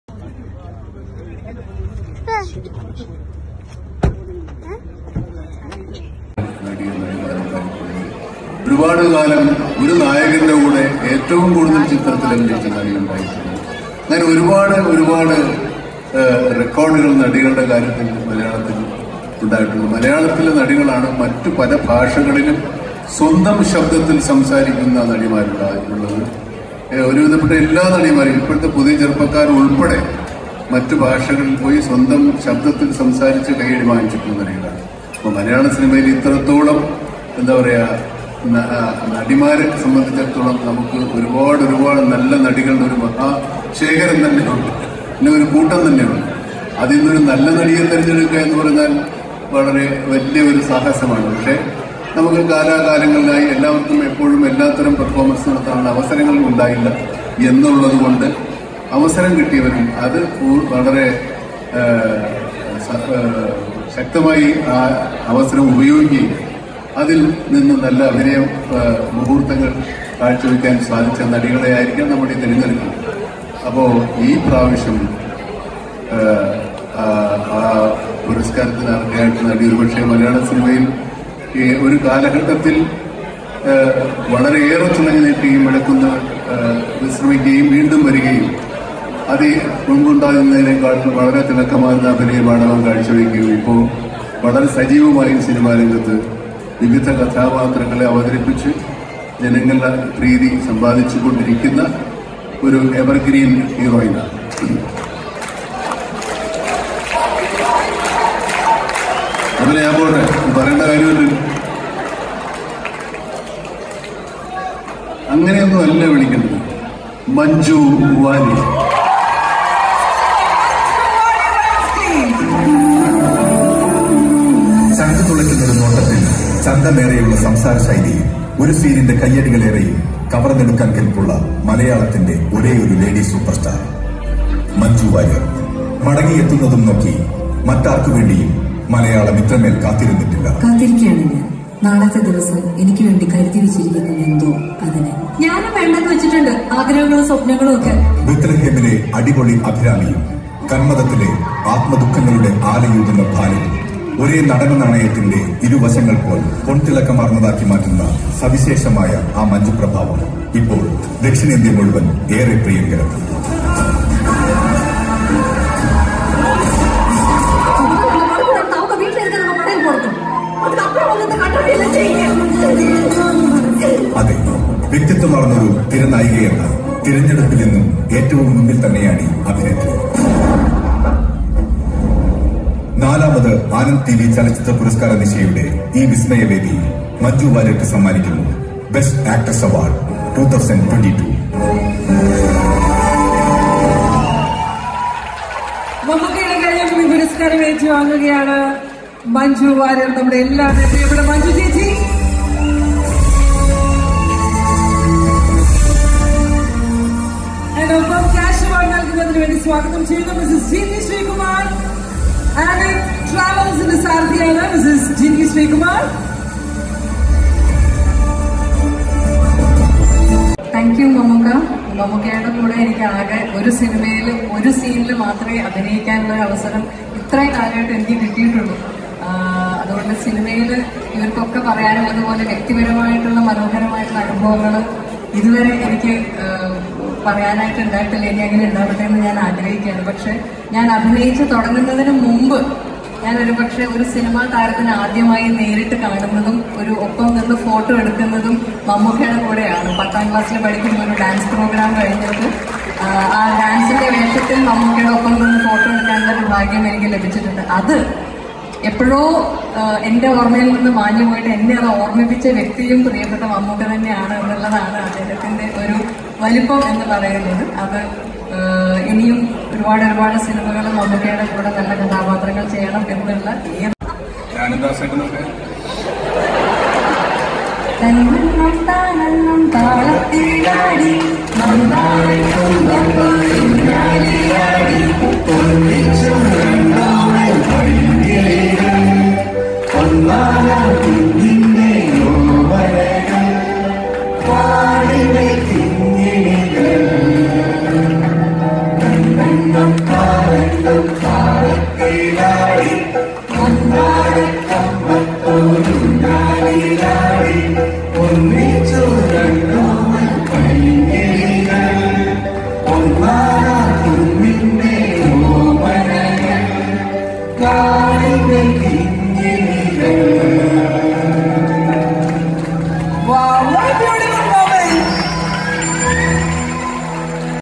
Best Actress2022
Manju Warrior receives best actress trophy from Mammukka